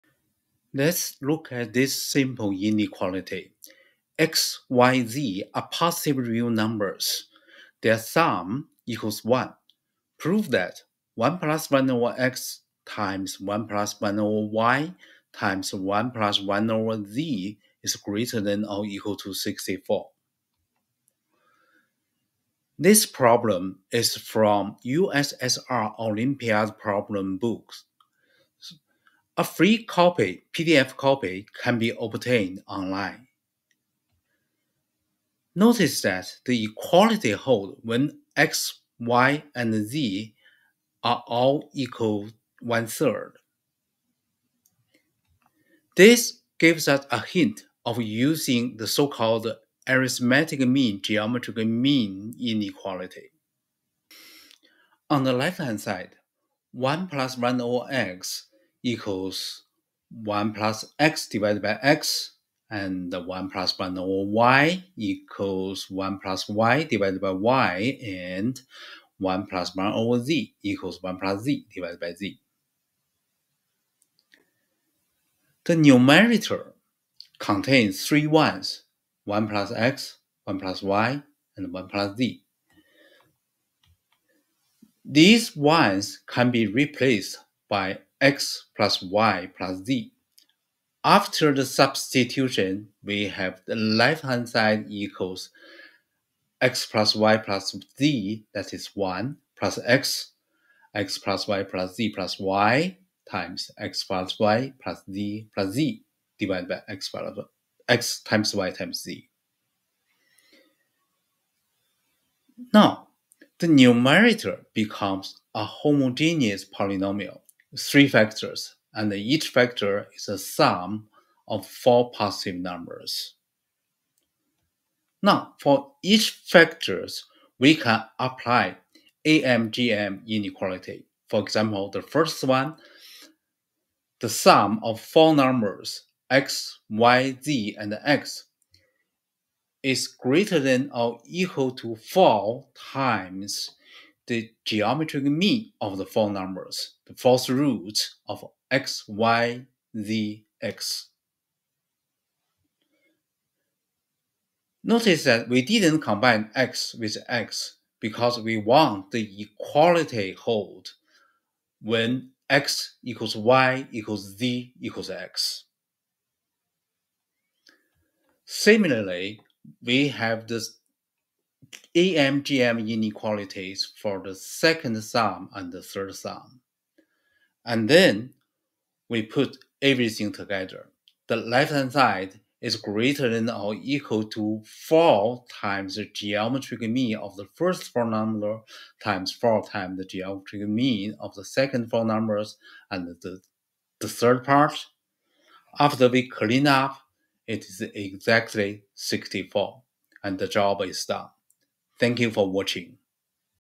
An Application of Arithmetic Mean Geometric Mean (AM-GM) Inequality
Arithmetic Mean Geometric Mean (AM-GM) Inequality tells that the arithmetic mean of some positive numbers is greater than or equal to their geometric mean. It is a useful tool to establish many other inequalities.